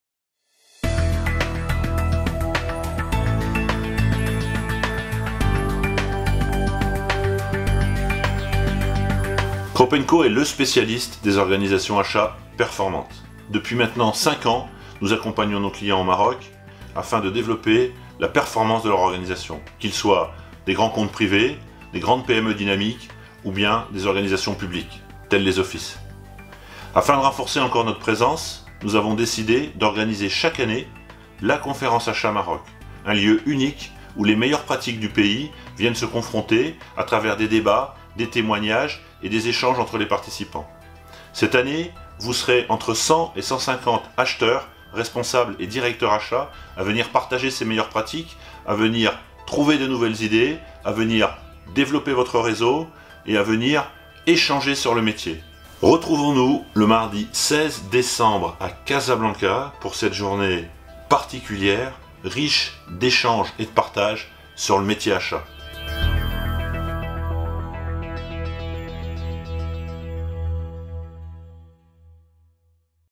16 décembre 2014 - La Conférence Achat Maroc - Casablanca - Crop and co
Le 16 décembre 2014 à Casablanca, Crop and co organise la seconde édition de La Conférence Achat Maroc.

"Les enjeux des organisations achats performantes"

Un événement unique au Maroc et une journée idéale pour :
- Découvrir les axes stratégiques des organisations achats performantes
- Rencontrer des expert achats internationaux
- Réaliser un benchmark des organisations achats performantes de votre secteur
- Développer votre réseau

Inscription gratuite sur présentation d'une invitation à télécharger sur le site Eventbrite dédié à l'événement